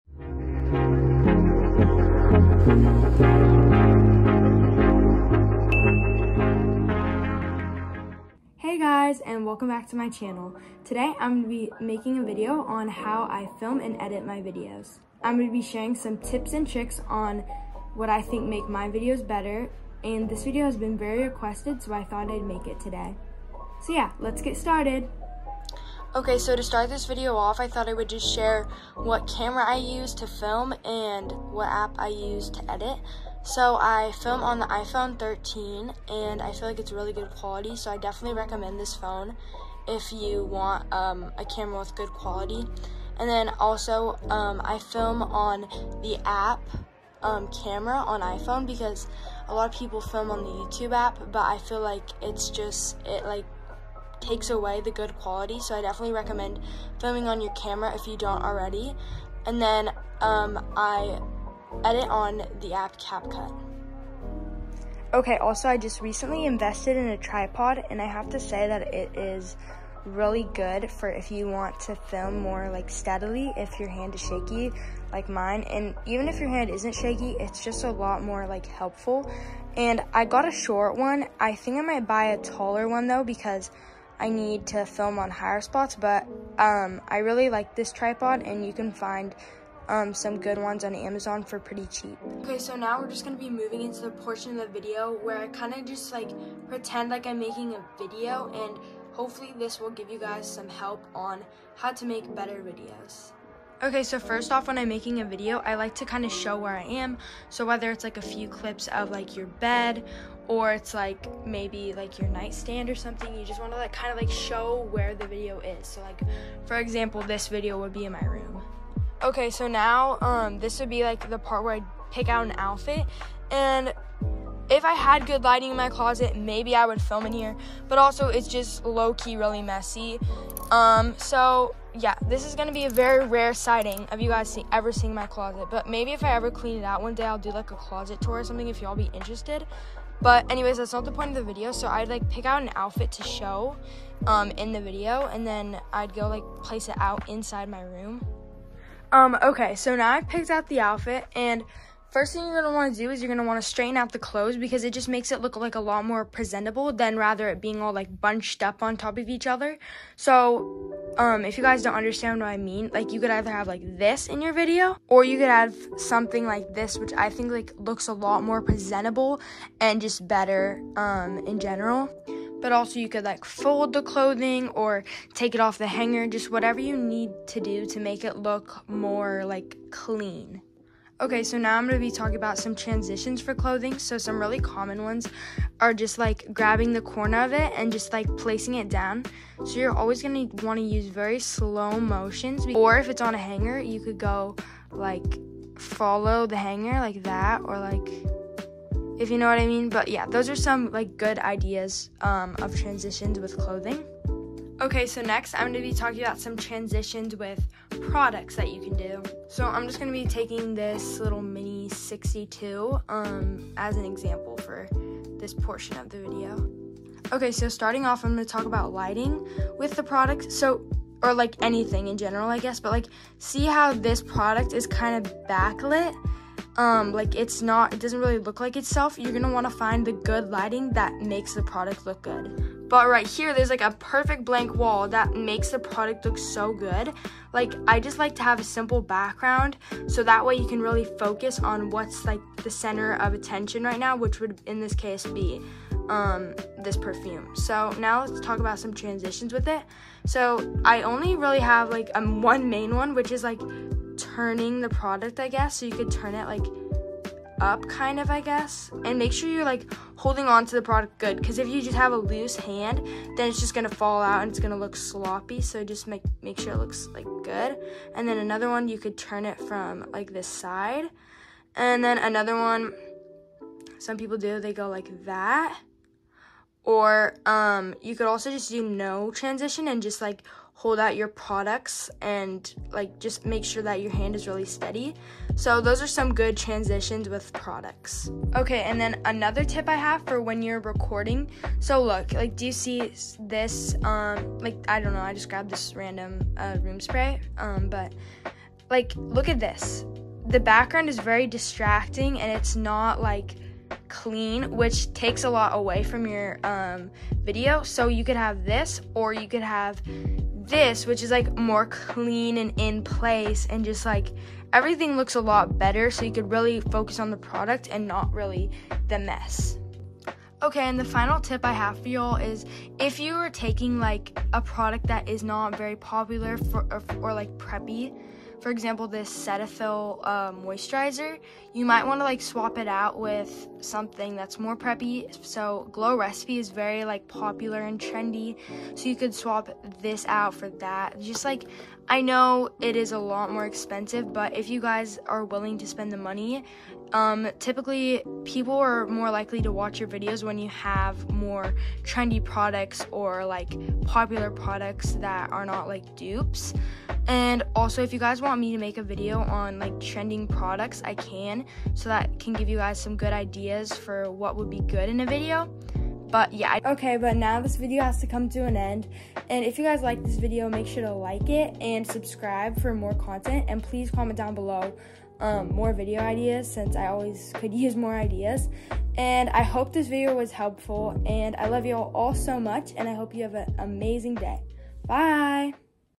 how i edit + make my videos! 🫶🏼☀️💐📸
this is a highly requested video so i hope yall enjoy it! if u see this comment the “📸” emoji and comment some video recommendations! love yall 🫶🏼🫶🏼